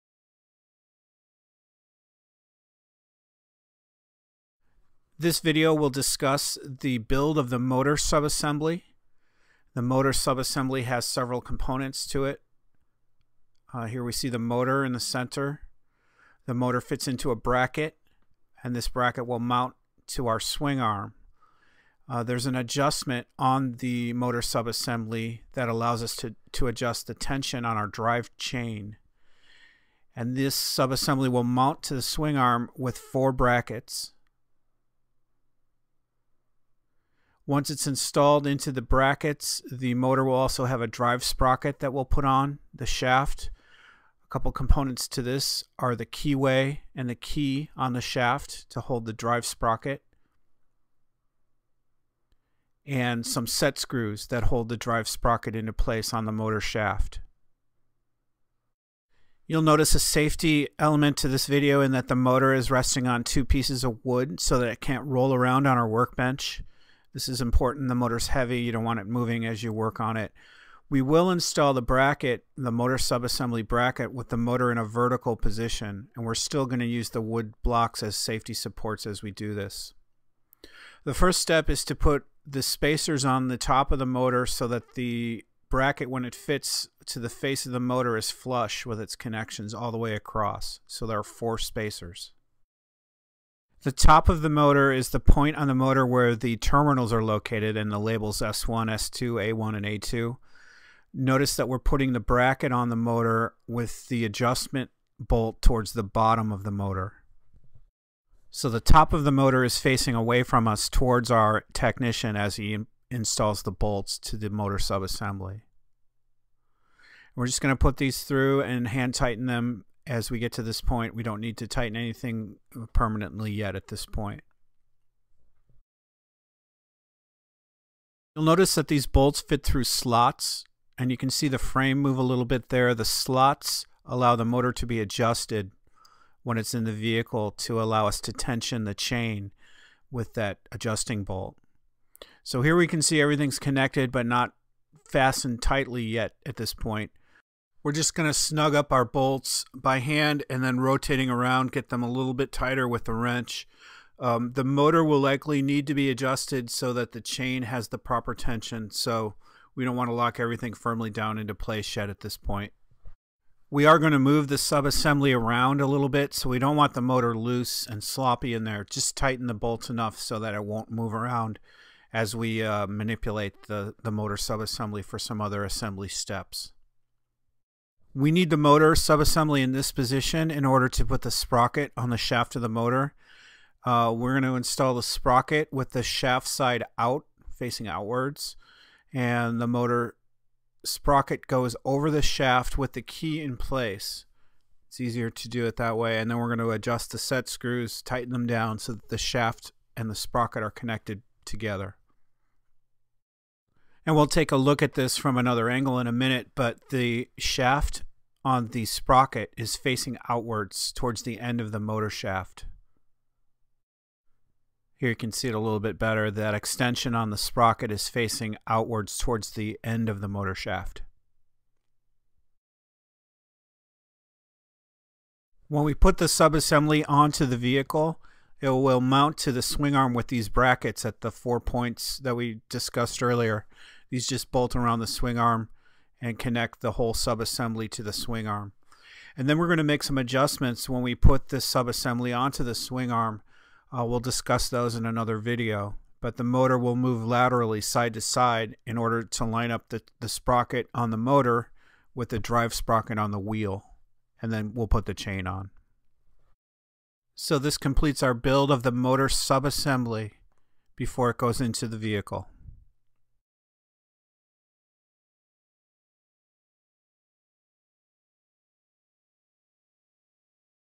Switch EV Motor Installation
How to install your motor into a Switch EV. Switch Vehicles is located in Sebastopol, CA. We build a sporty, low cost electric vehicle.  Every design decision was made to meet four criteria: safety, ecology & economy, beauty